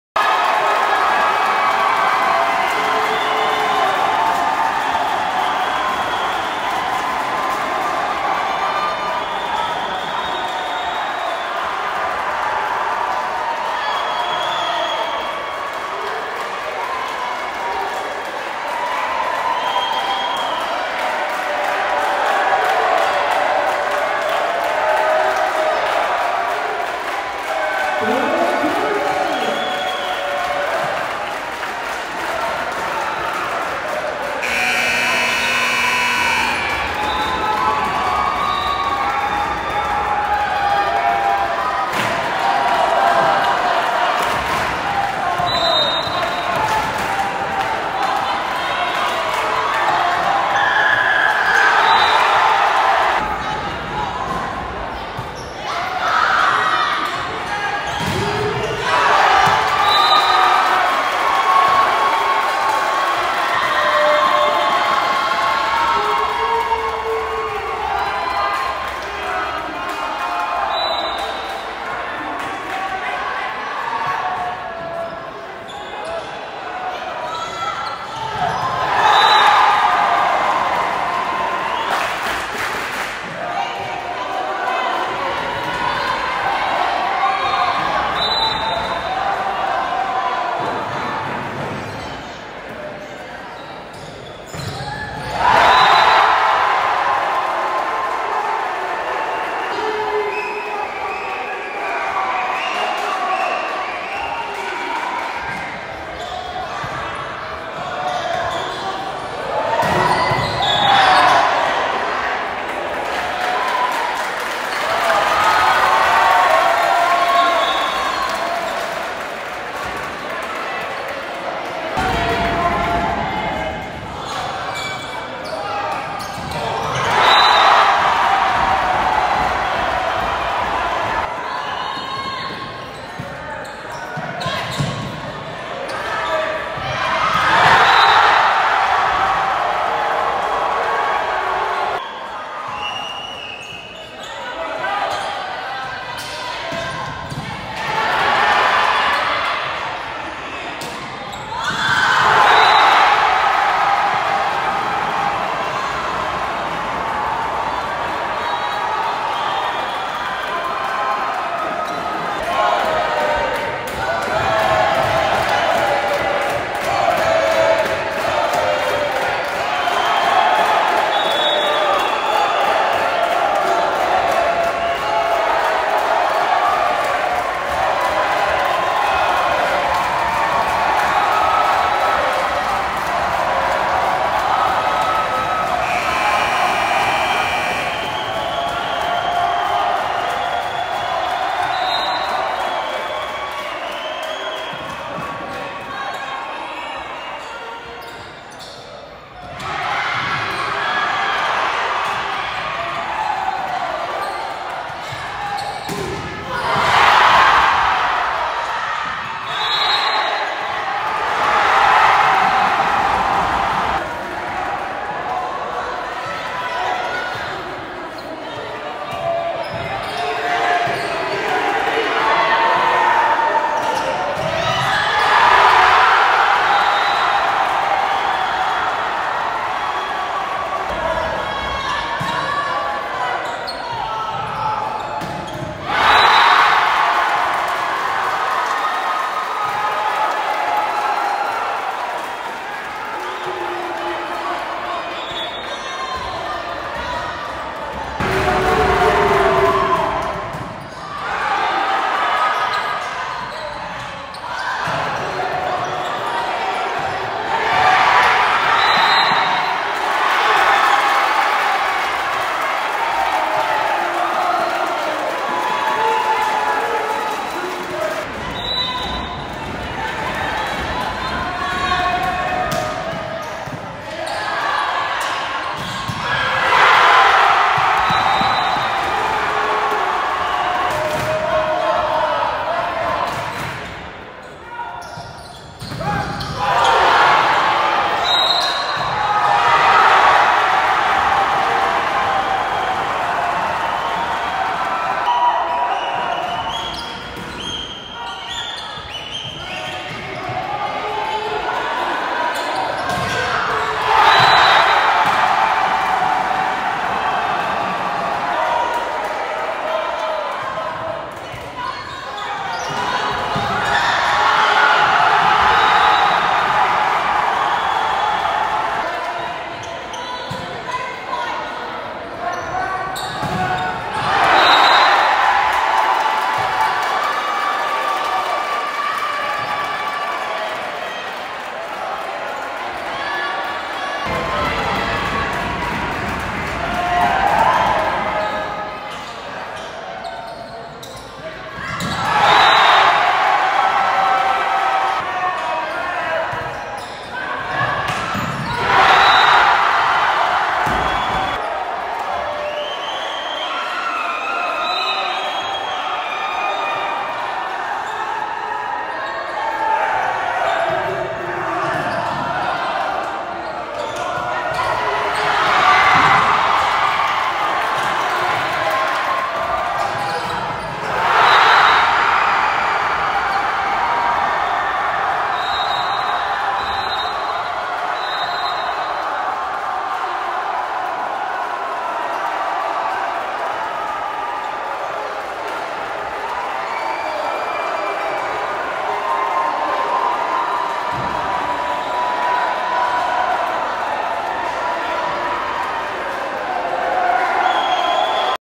Seminole Ridge vs Park Vista. 5/10/22. Regional Finals. Set 5
Final score:
5-15. Park Vista won the 5th set
Overall for game:
2-3. Park Vista won the Regional Finals